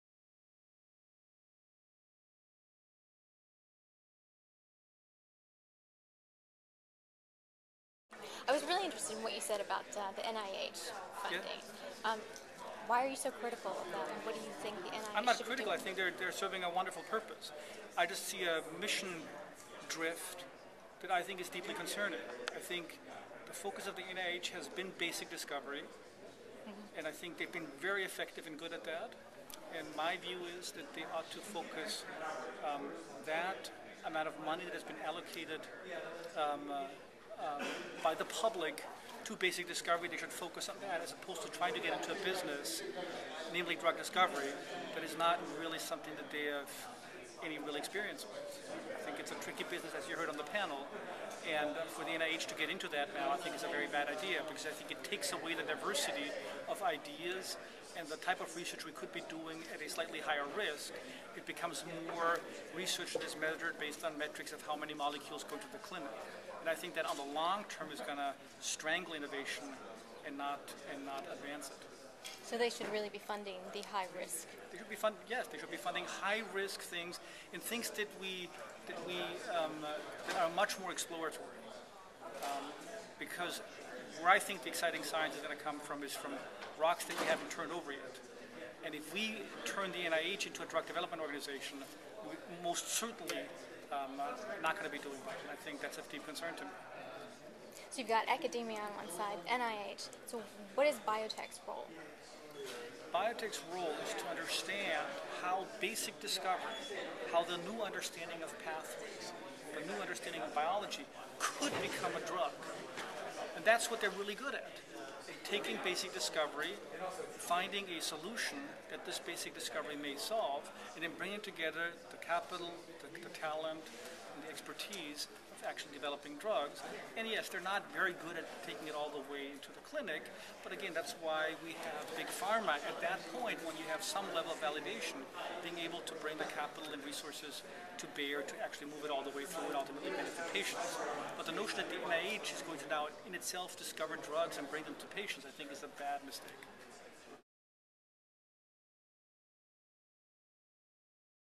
2011 BIO International Convention - Interview with Tillman Gerngross
BioWorld grants you an exclusive interview with Tillman Gerngross, Fo-Founder and CEO of Adimab. He reviews his session at BIO entitled, "Disco Inferno - Pharma Feels the Heat with Discovery Deals."